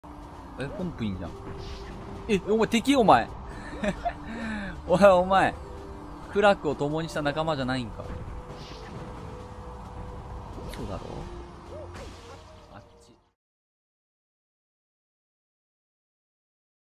オヤ・マーは中立なのか？黒幕なのか？
プレイ時は気づかなかったですが
オヤ・マーとGALAXYの繋がり！